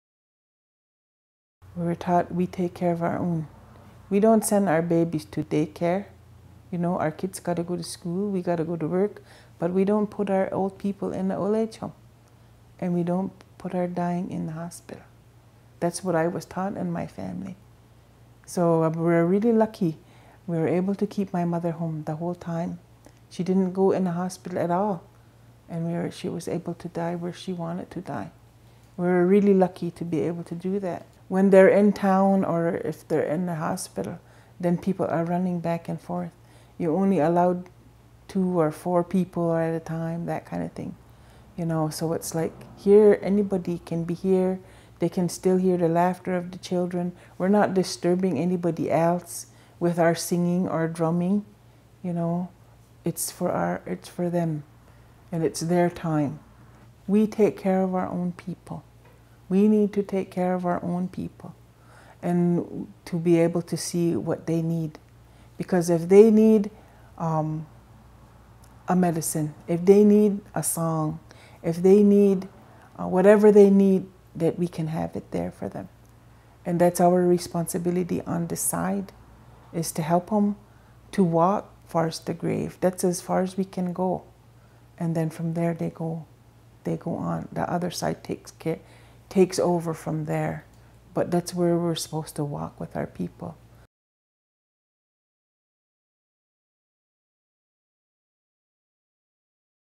Elder Elva: Dying at home
In this video by Canadian Virtual Hospice, Elder Elva Jameson describes the importance of dying at home.